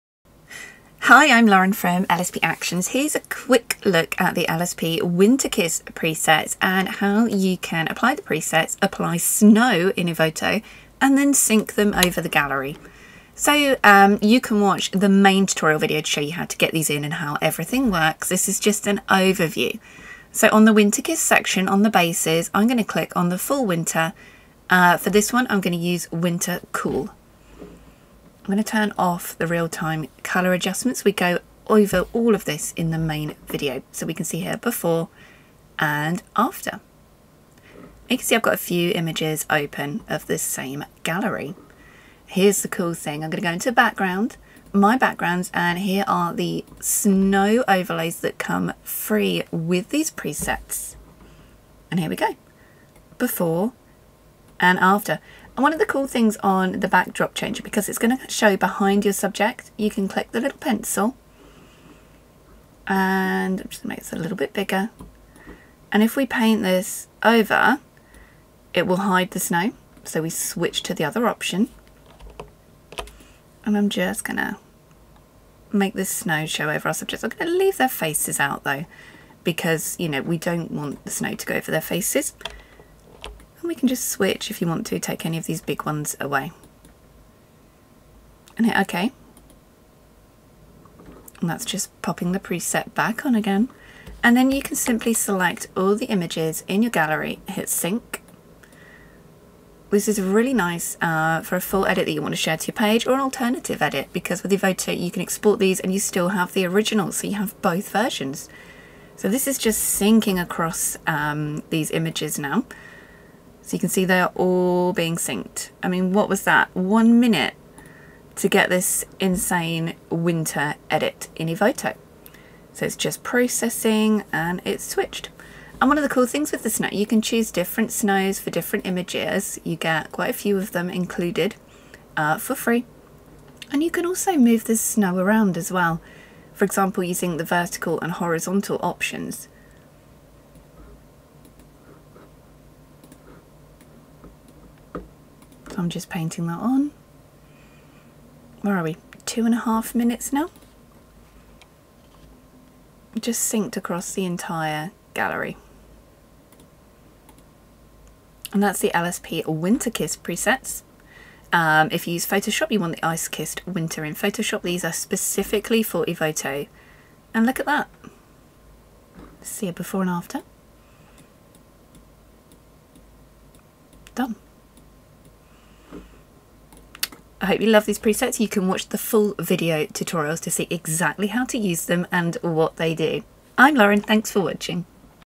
Winter Kiss Editing in Evoto Tutorial: A full winter snow gallery edit with gorgeous finish
How to edit a gorgeous winter scene with the LSP Winter Kiss Evoto Pack to create a gorgeous frosty winter edit

👨‍👩‍👧 Presets Used: LSP Evoto Winter Kiss Presets
💻 Software: Evoto.ai